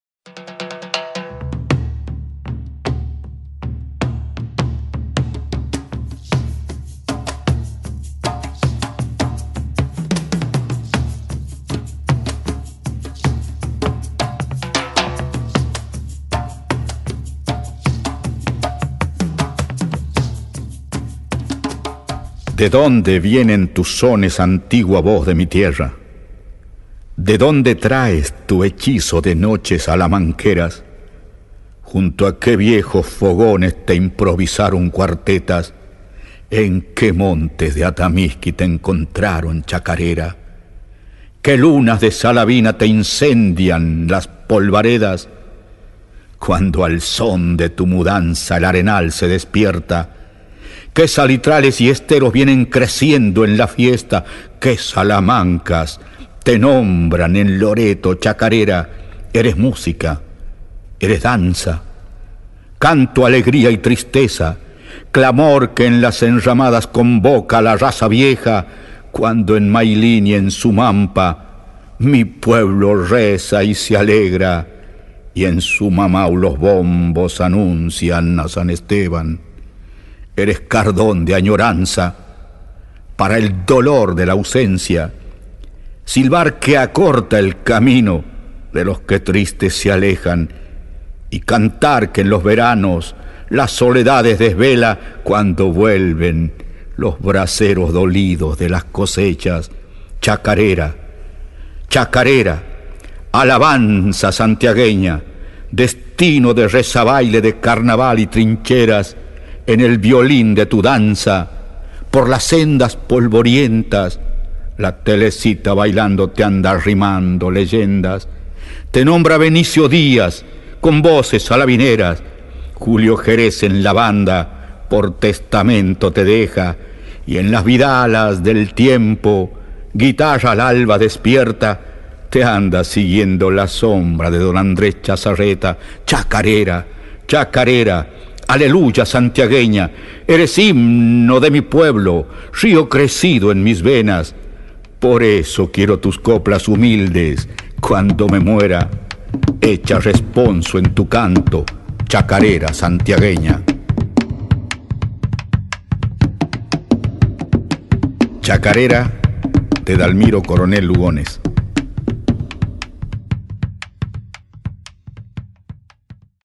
CHACARERA de Dalmiro Coronel Lugones por Alfonso Nassif
CHACARERA
De Dalmiro Coronel Lugones

De donde vienen tus sones
Antigua voz de mi tierra,
De donde traes tu hechizo
De noche salamanquera,
Junto a qué viejos fogones
Te Improvisaron cuartetas,
En que montes de Atamisqui
Te encontraron, chacarera

Que lunas de Salavina
Te incendian las polvaredas
Cuando al son de tus mudanzas
El arenal se despierta,
Que salitrales y esteros
Vienen creciendo en tu fiesta,
Que Salamancas te nombran
En Loreto Chacarera..?

Eres música, eres danza,
Canto, alegría y tristeza,
Clamor que en las enramadas-
Convoca a la raza vieja
Cunado en Mailín y en Sumampa
Mi pueblo reza y se alegra
Y en Sumamao los bombos
anuncian a San Esteban...

Eres cardón de añoranzas
Para el dolor de la ausencia,
Silbar que acorta el camino
De los que tristes se alejan
Y cantar que en los veranos
Las soledades desvela
Cuando vuelven los braceros
Dolidos de las cosechas...

Chacarera, chacarera
Alabanza santiagueña
Destino de rezabailes,
De carnaval y trincheras
En el violín de tu danza
Por las sendas polvorientas
La Telesita bailando
Te anda arrimando leyendas...

Te nombra Benicio Díaz
Con voces salavineras
Julio Jerez en La Banda
Por testamento te deja,
Y en las vidalas del tiempo
-- Guitarra al alba despierta --
Te anda siguiendo la sombra
De don Andrés Chazarreta...!

Chacarera, chacarera
Aleluya santiagueña,
Eres himno de mi pueblo,
Río crecido en mis venas,
Por eso quiero cantarte
Con la emoción de un poema
Y el corazón entregarte
En esta lírica ofrenda...

Por eso quiero tus coplas
Humildes cuando me muera,
Quiero tu música criolla
Sonando triste en mi, ausencia
Para sentir que me llevo
El alma de mi querencia
Hecha responso en tu canto
Chacarera santiagueña...!